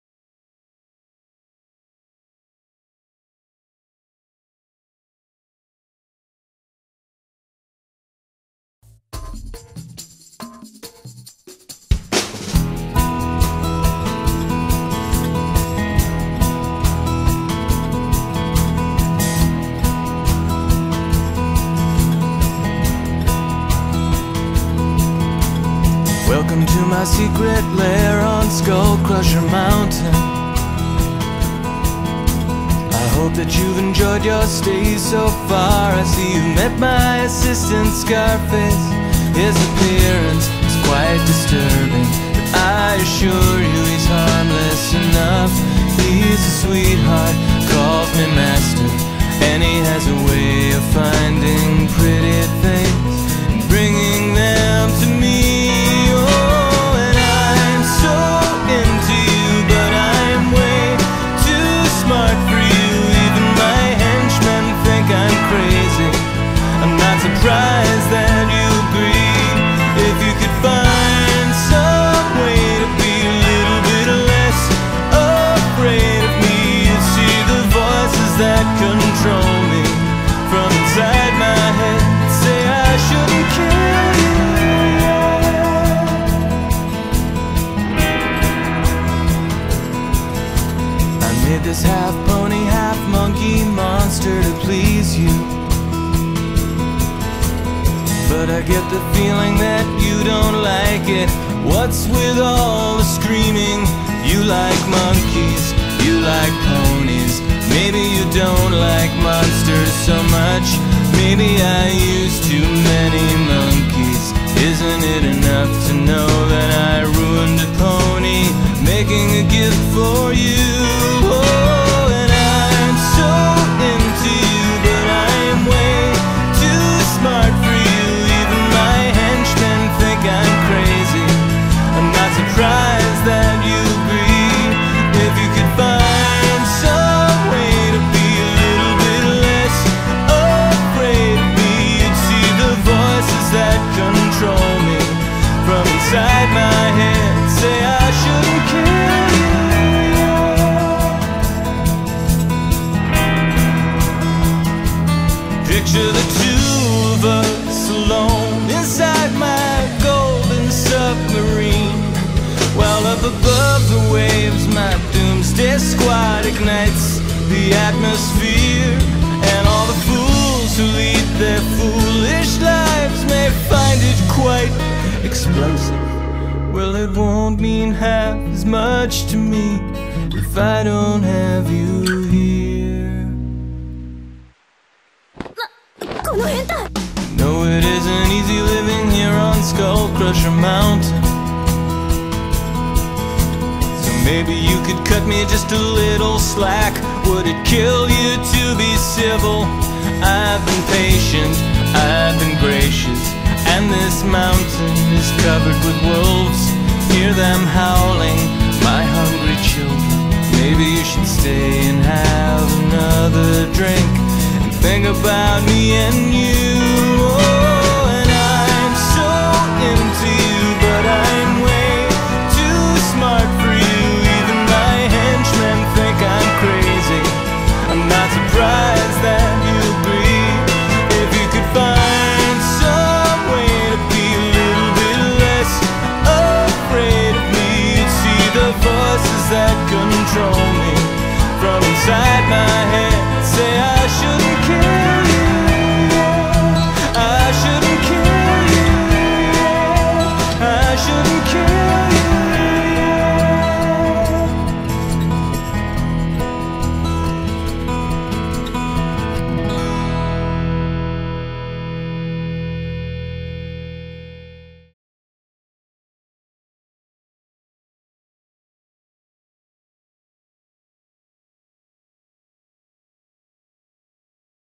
The Mad Scientist in Love
My entry for the 2013 Dragon*Con Fandom Music Video Contest (Anime: Romance category)

by LEONHART Anime Music Videos